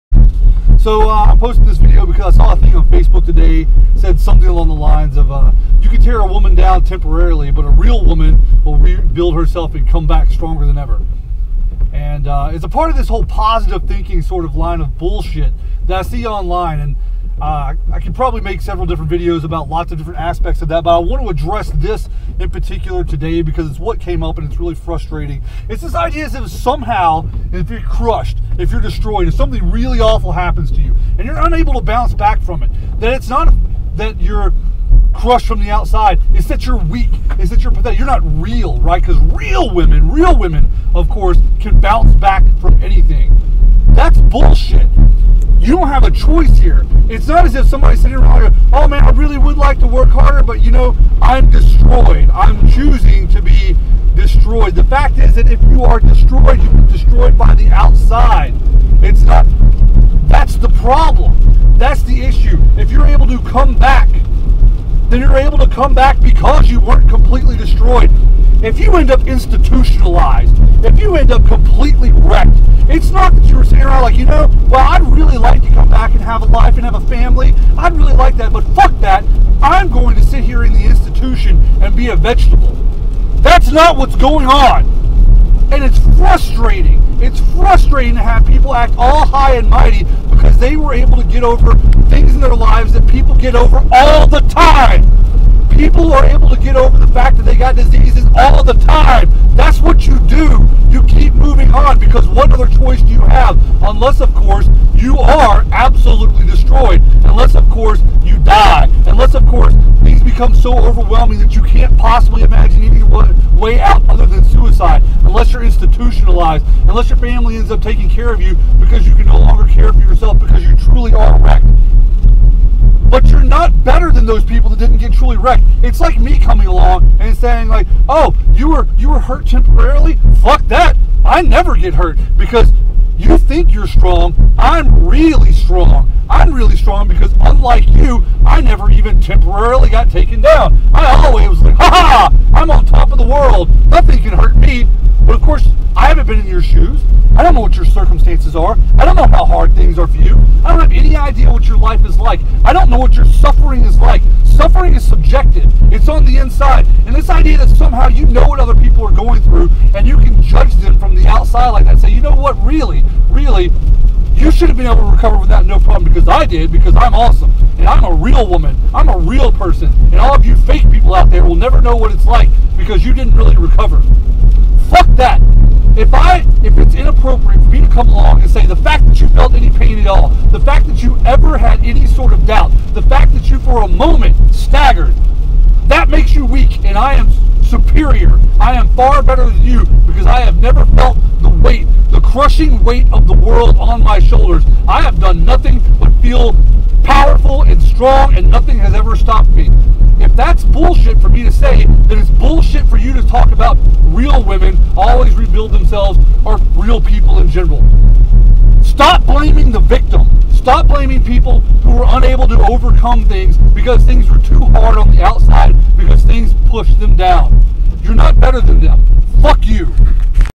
On the Idea that "Real" People are Never Completely Destroyed
I saw an image on Facebook that read "You can break down a woman temporarily, but a real woman will always pick up the pieces, rebuild herself, and come back stronger than ever." The implication is clearly that if you are unable to do this, you are not "real," you're some kind of fake person. That's bullshit.